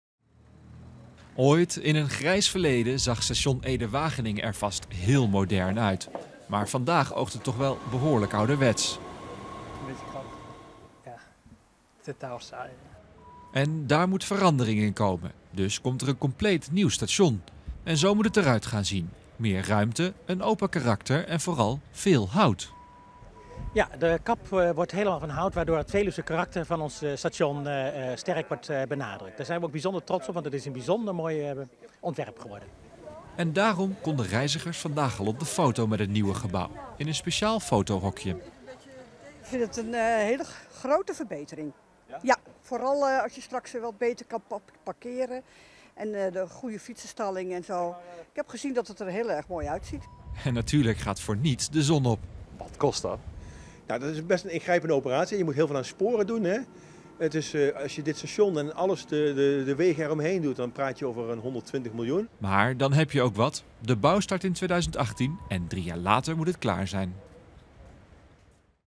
Zo gaat het nieuwe station Ede-Wageningen eruit zien - Omroep Gelderland
Het nieuwe station Ede-Wageningen wordt het grootste houten gebouw van Nederland. De gemeente Ede, ProRail en de NS presenteerden donderdagavond het ontwerp voor het nieuw te bouwen station.